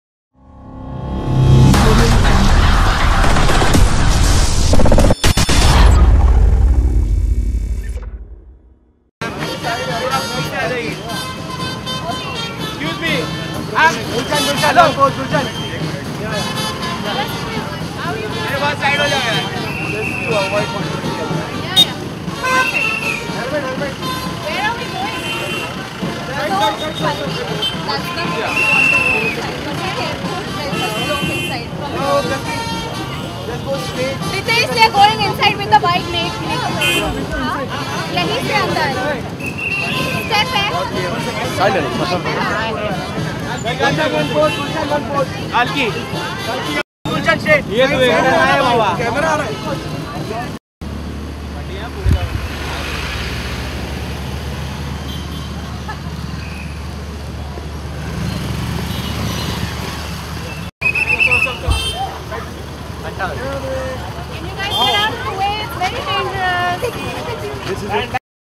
Kalki Koechlin And Gulshan Devaiya Bike Ride On Goa Street
Be a part of the Screen world and know your Stars personally, their journey, be a part of their ups-downs, and enjoy the greatest film Kalki Koechlin And Gulshan Devaiya Bike Ride On Goa Street
producing experience from 'BOLLYWOOD ADDA'